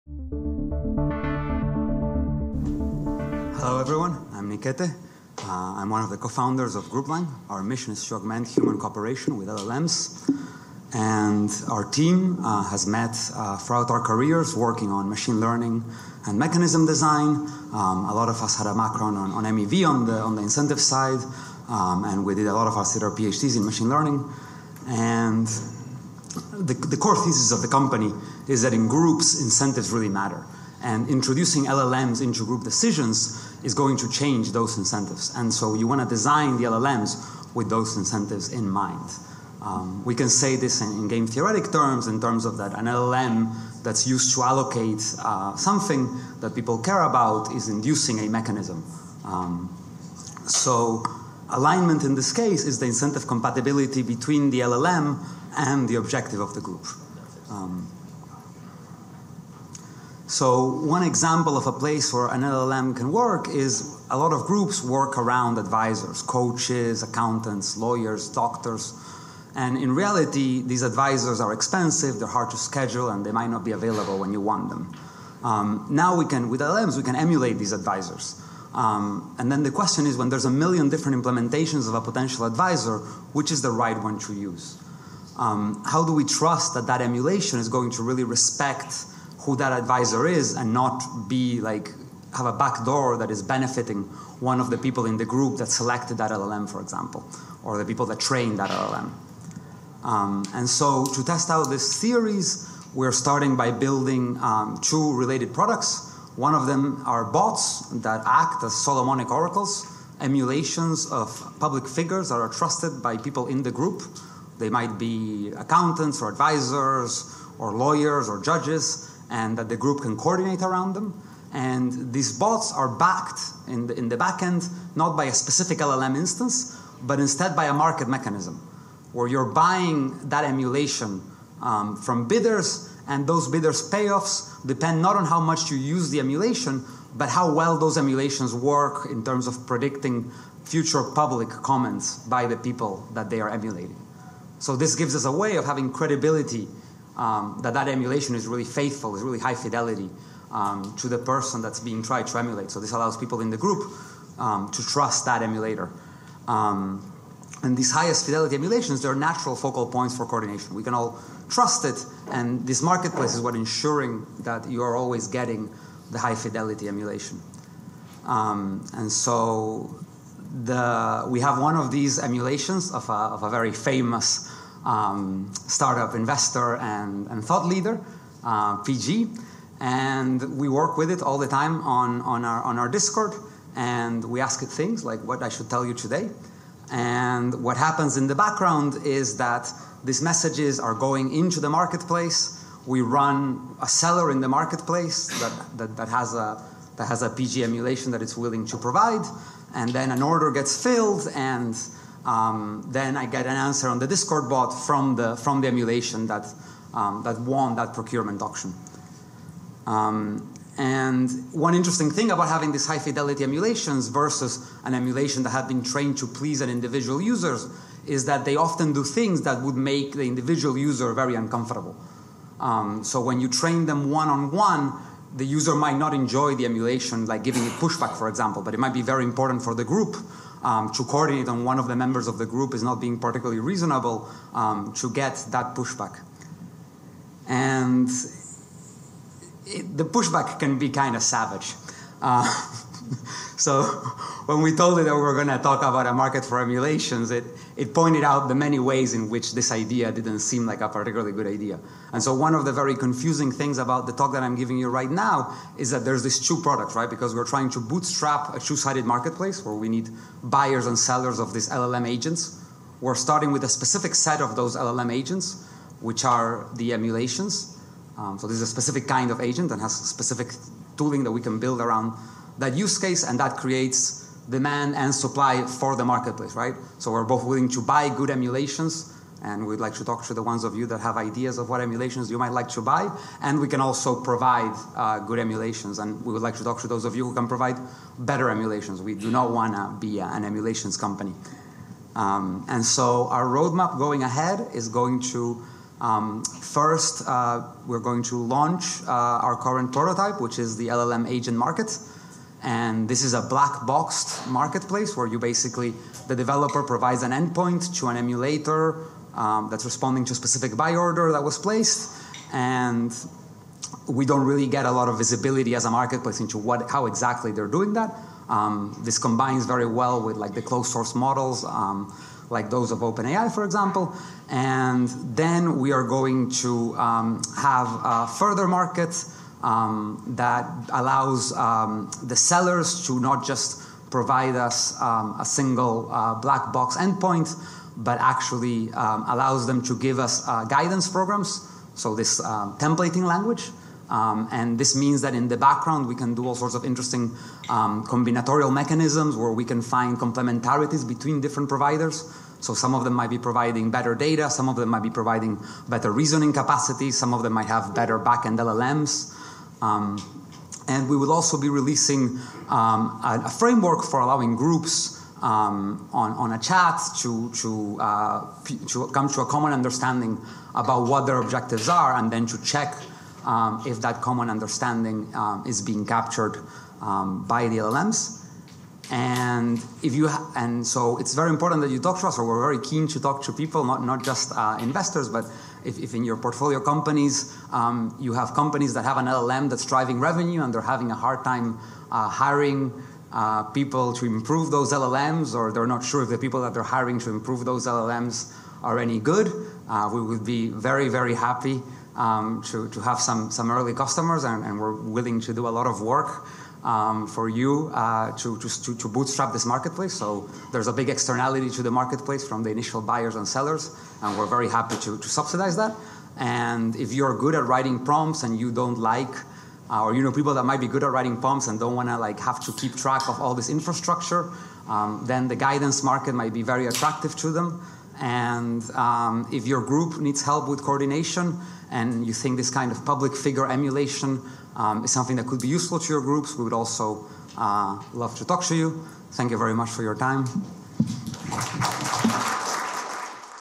GroupLang – Demo at AI Camp: Augment
GroupLang is building software that tackles the way that LLMs interact with groups of people, rather than individuals. Founder Nikete della Penna and Bruno Mazorra understand that alignment of LLMs to a single, well-defined set of preferences is challenging enough on its own, and that same alignment with a group of people requires serving less well-defined preferences and yields fundamental incentive constraints. The company is tackling the big questions of how an LLM can serve a group and learn from a group.